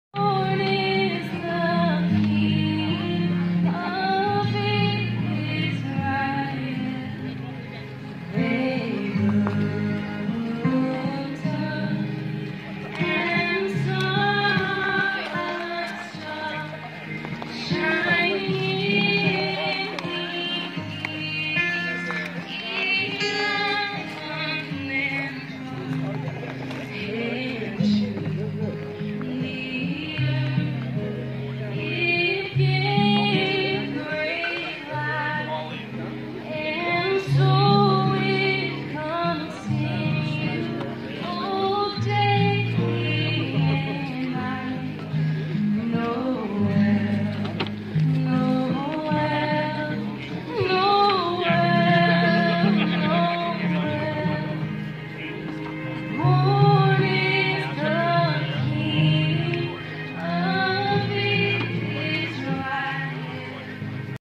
Live Nativity in Sherborn
Paige Carroll performed holiday favorites just before the annual presentation of the Live Nativity at the corner of Washington Street and Western Avenue in Sherborn on Dec. 22 and 23.